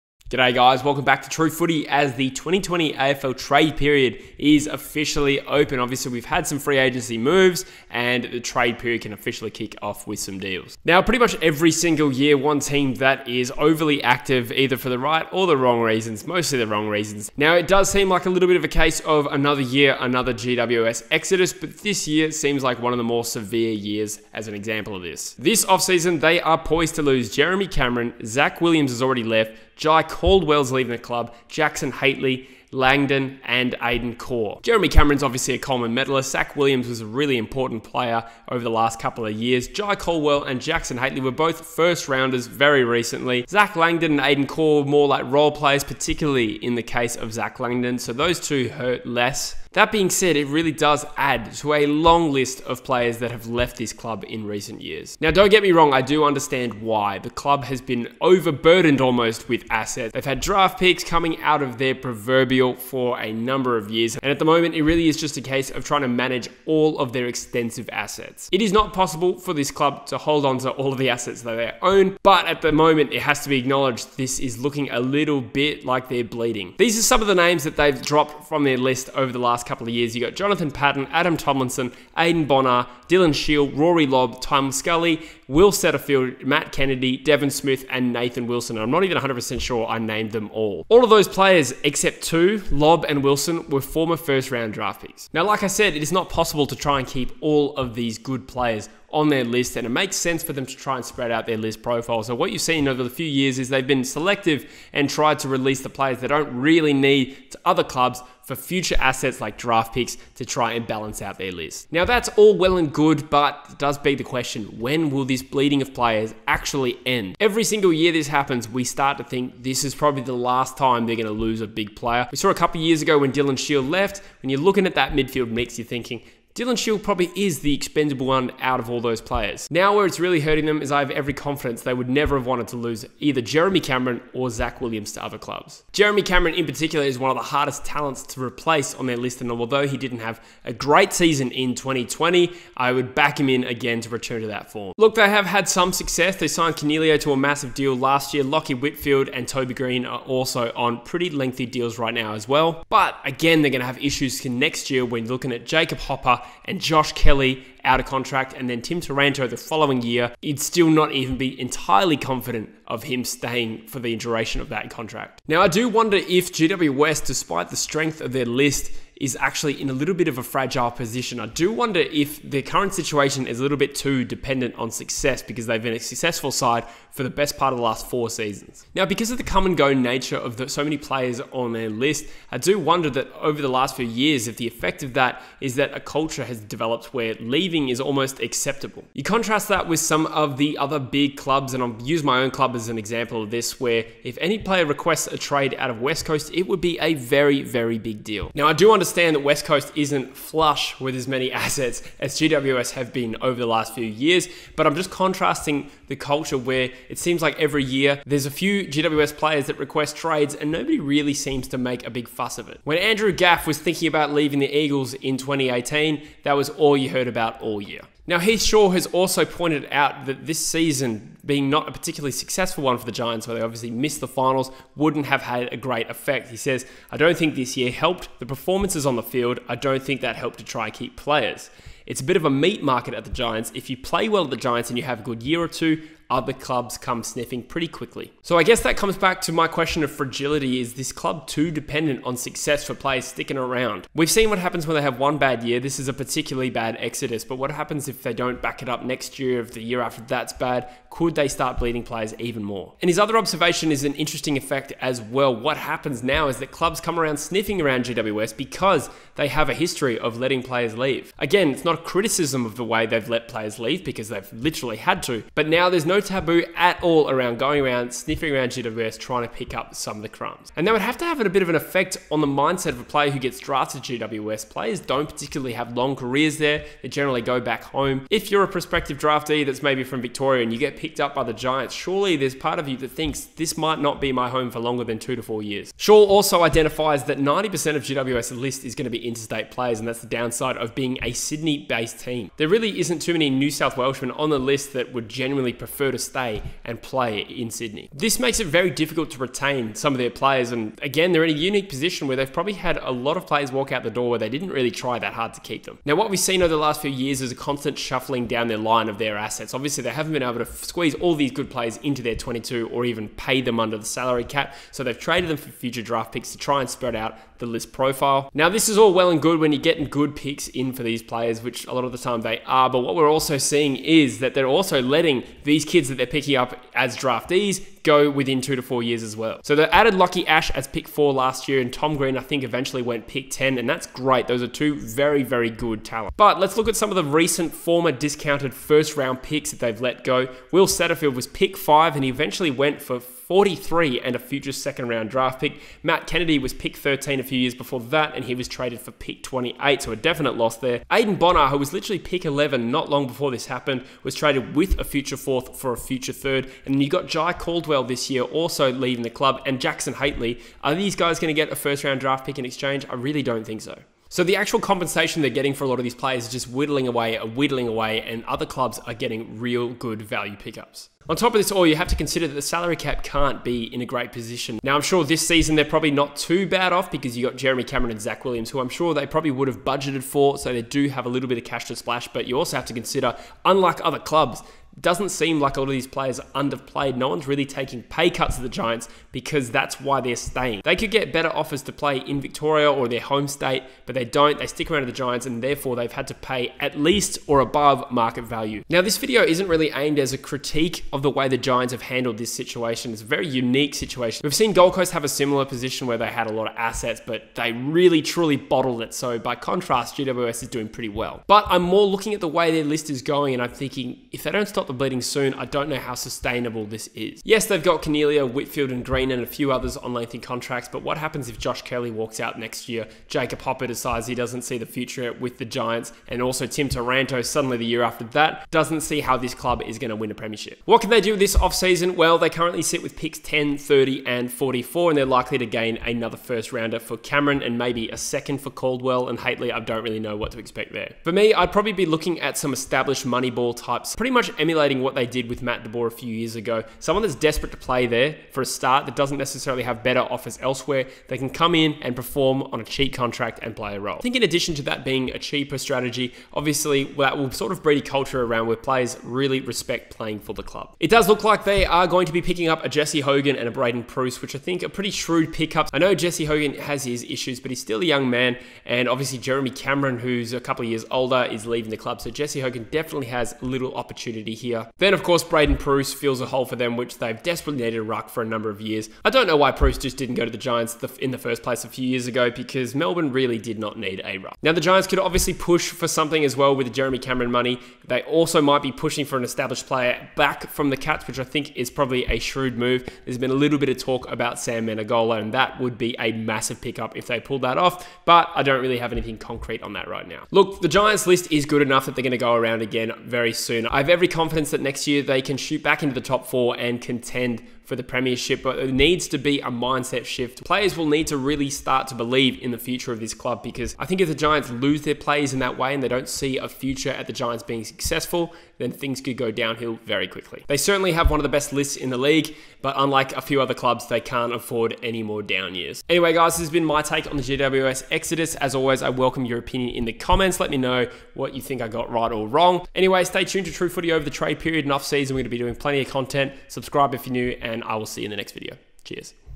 AFL TRADES: Can the GWS Giants stop the exodus?
As the AFL trade period begins, the GWS Giants are set to lose stars such as Jeremy Cameron and Zac Williams, in addition to Jackson Hately, Jye Caldwell, Aidon Corr and Zac Langdon.